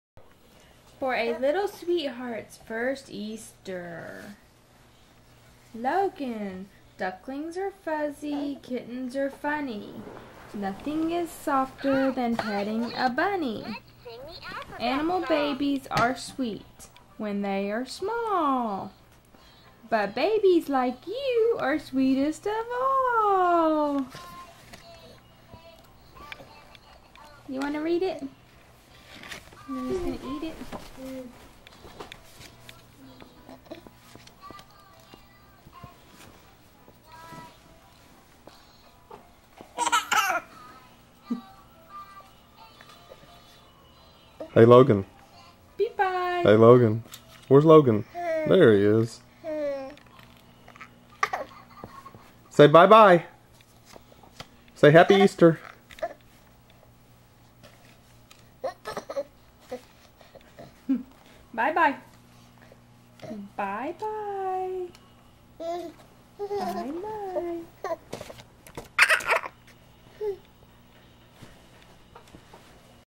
Logan's First Easter Card
Baby Logan reads and chews on his first Easter card, from his grandparents, March 22, 2008.